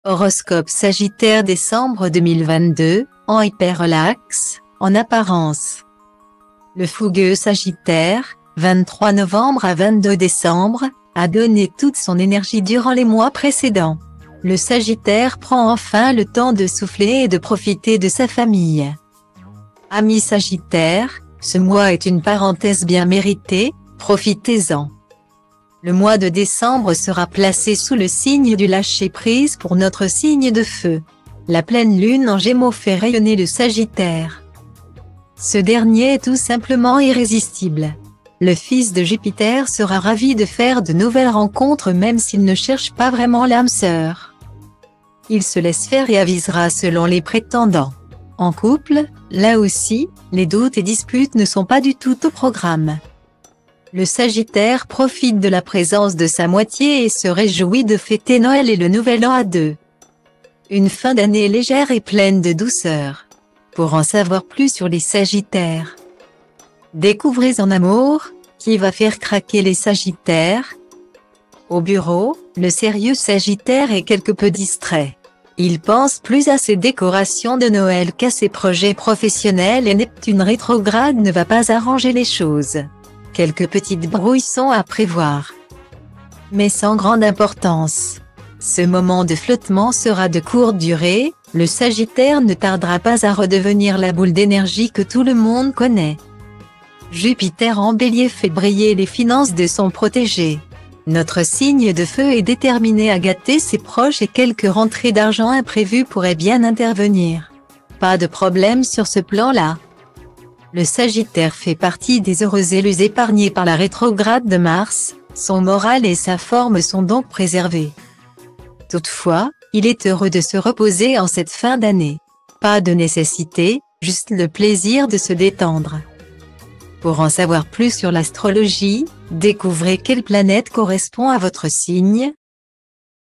Horoscope Sagittaire décembre 2022 : en hyper relax… en apparence !
Le fougueux Sagittaire (23 novembre-22 décembre) a donné toute son énergie durant les mois précédents ! Le Sagittaire prend enfin le temps de souffler et de profiter de sa famille.Amis Sagittaire, ce mois est une parenthèse bien méritée, profitez-en !Le mois de décembre sera placé sous le signe du lâcher-prise pour notre signe de Feu ! La Pleine Lune en Gémeaux fait rayonner le Sagittaire,